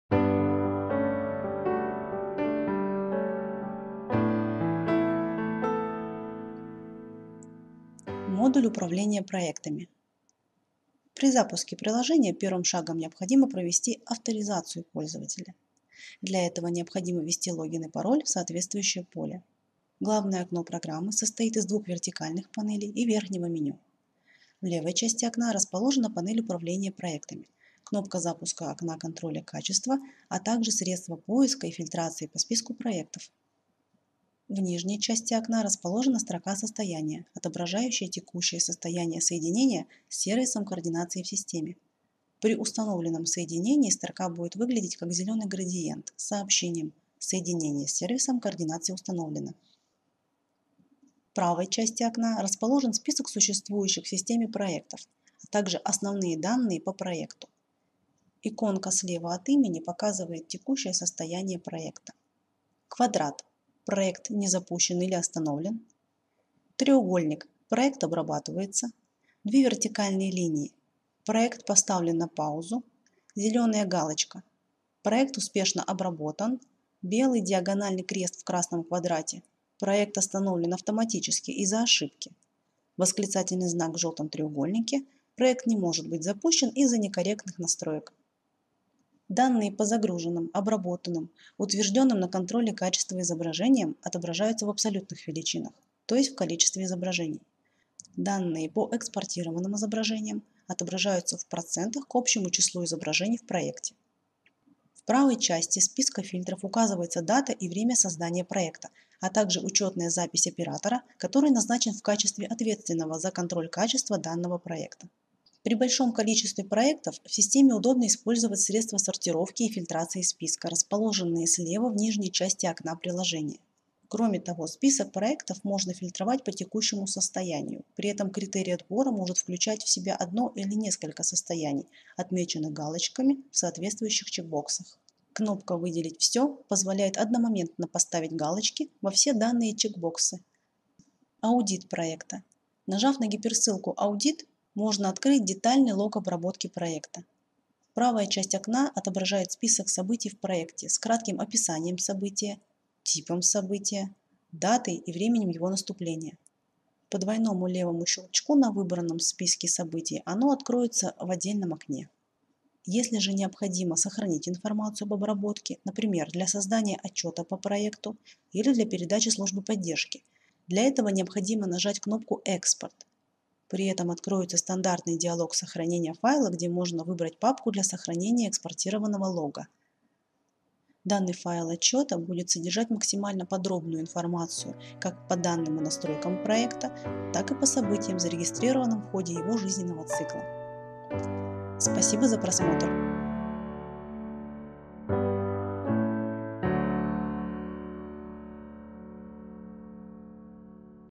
ALANIS BSP (Book Scan Processing). Описание модуля управления проектами
Ролик знакомит с Модулем управления проектами, основным пользовательским модулем поточной системы обработки книжных сканов ALANIS Book Scan Processing.
Видео демонстрирует пользовательский интерфейс, а также дает обзор функций модуля мониторингу обработки проектов обработки.

ALANIS Book Scan Processing - мощная распределенная система потоковой обработки книжных сканов с широким набором функций улучшения качества графических образов. Система создана в сотрудничестве с Российской Государственной Библиотекой и успешно используется специалистами библиотеки с 2016 г.